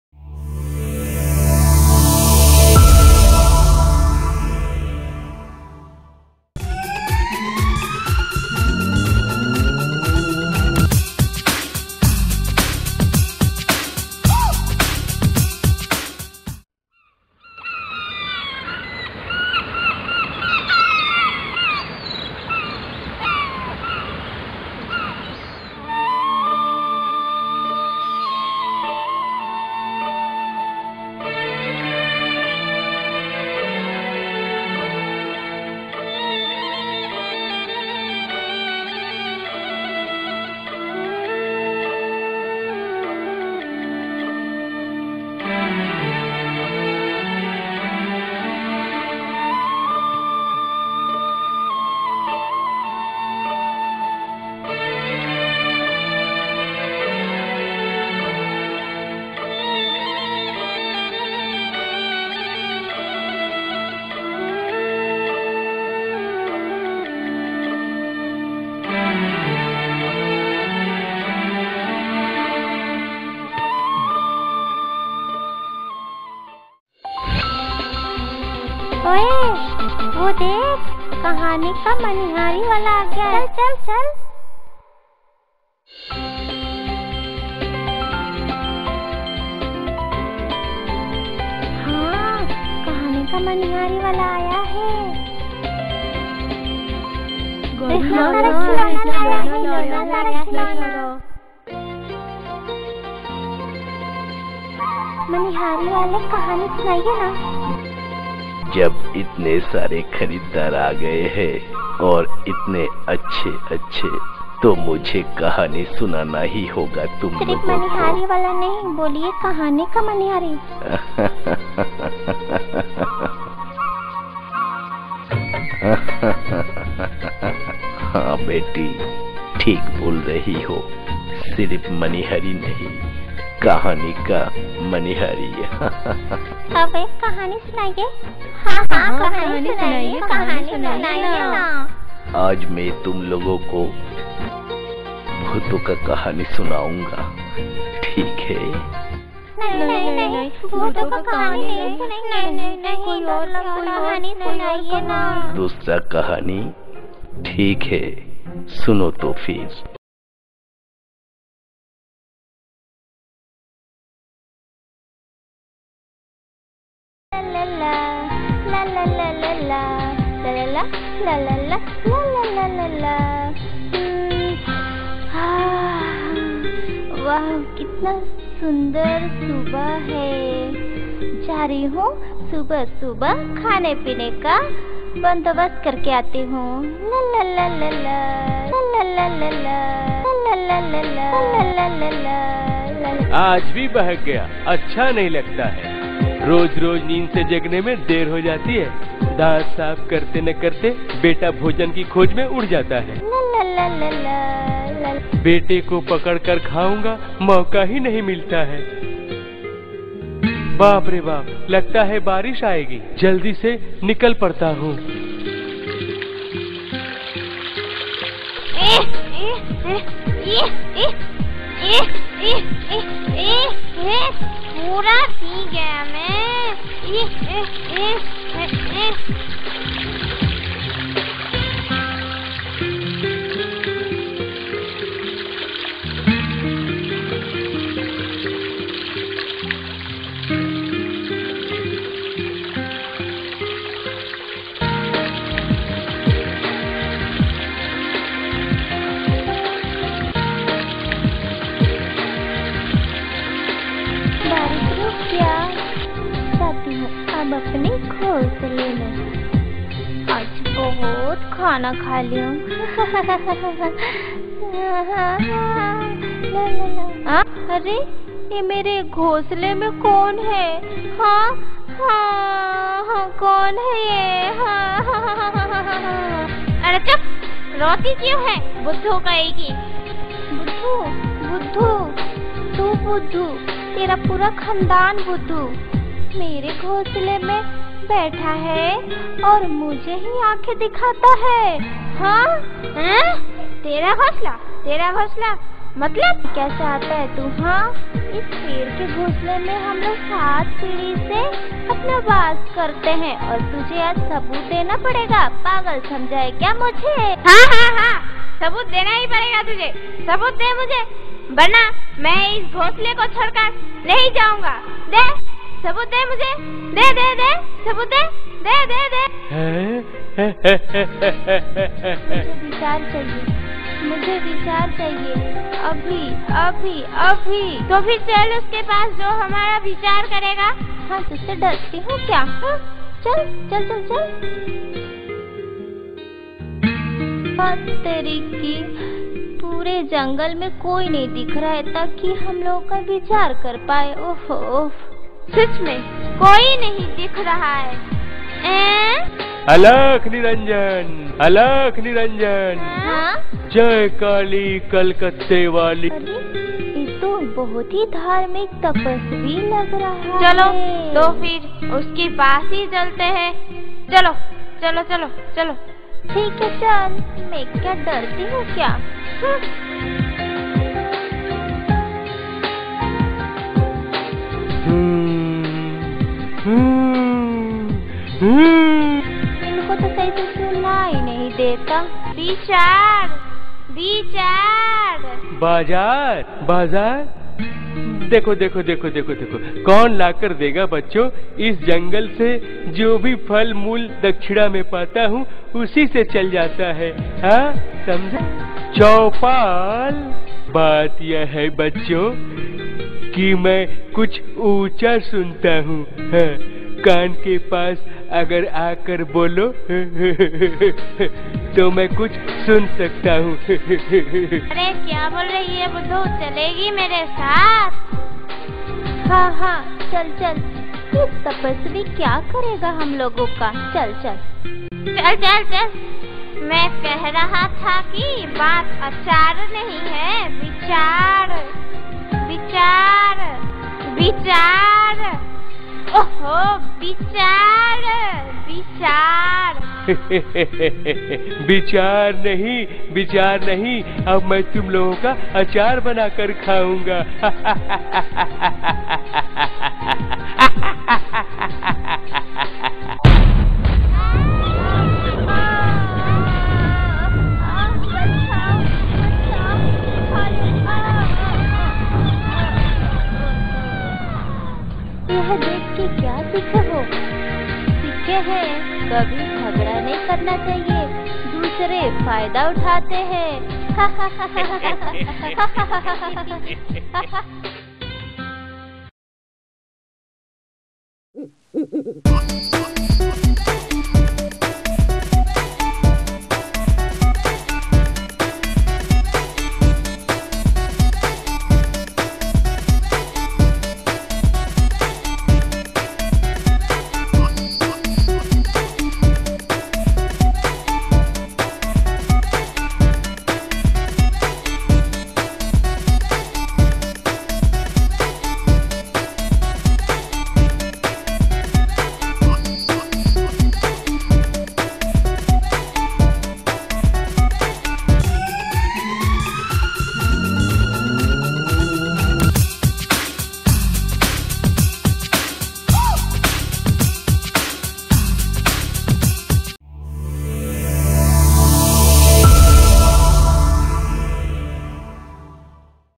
Dushta Billi Aur Khargosh | Hindi story for children | Panchatantra Kahaniya | Stories of Kananmama
Dushta Billi Aur Khargosh (दुष्ट बिल्ली और खरगोश) ::
A cat and a parrot lived in a forest. One day when the parrot went in search of food it started raining heavily and a rabbit took shelter in his house and they are quarreled for shelter. When cat disguised as a sage met them on the way and devoured them. The moral of the story is that we should never fight and let others take advantage of us.